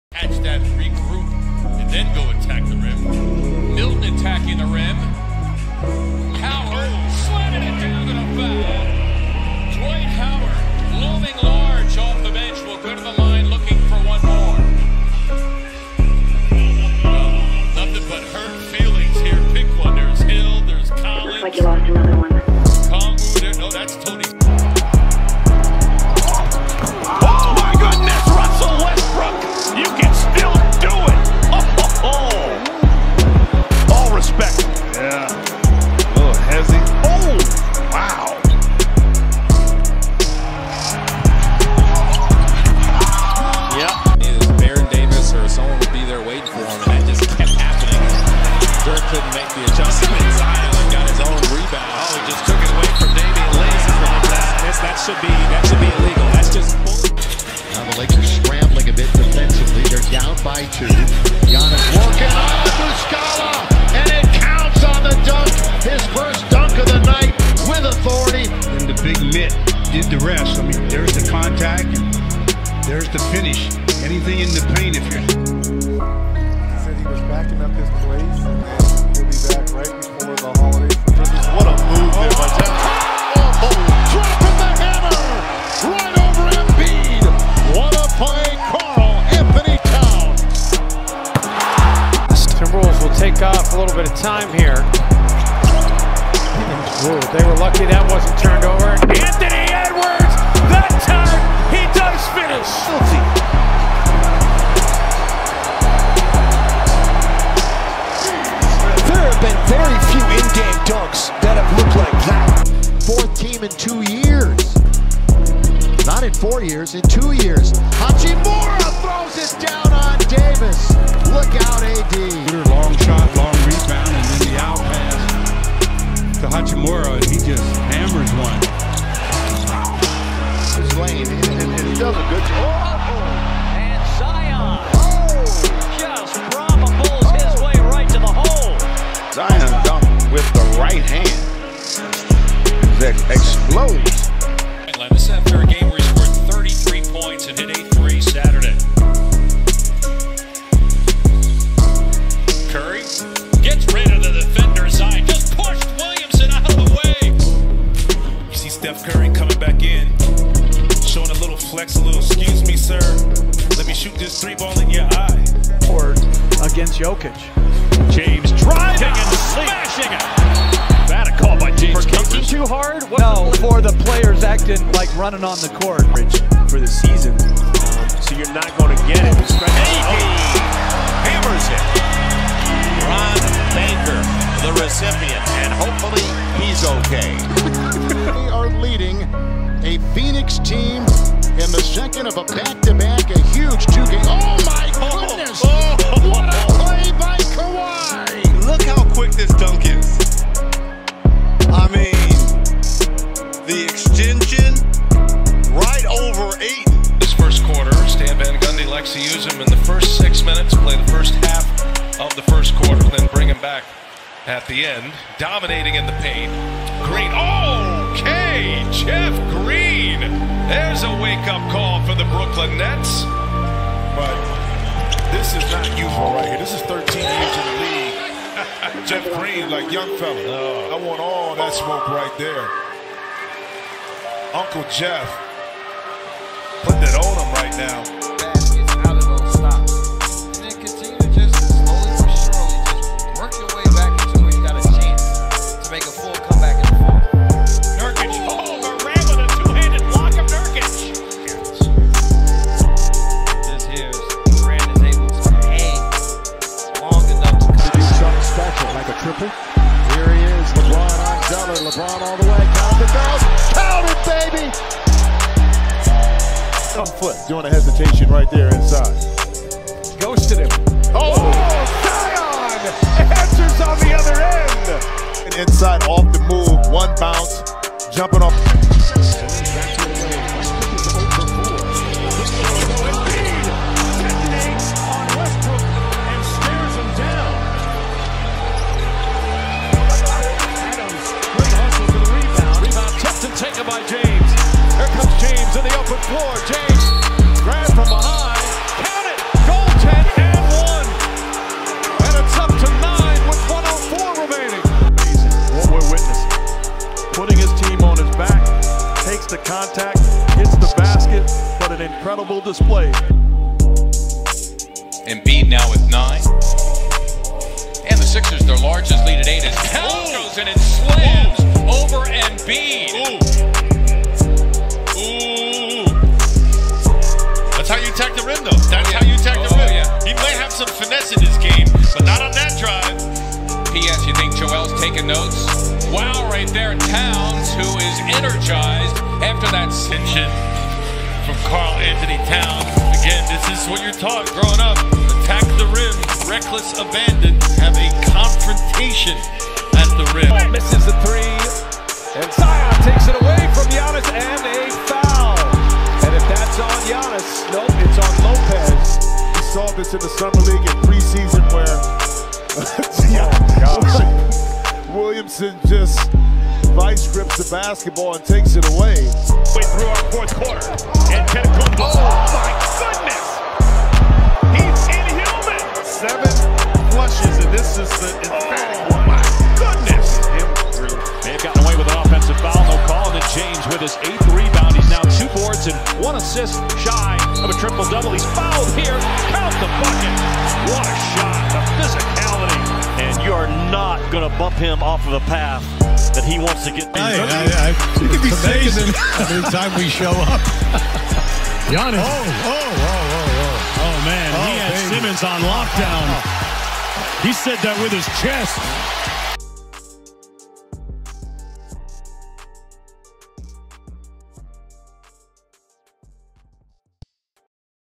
NBA "STRONGEST!!!" MOMENTS
When NBA players show their physicality and dominates the game...

Music from anarky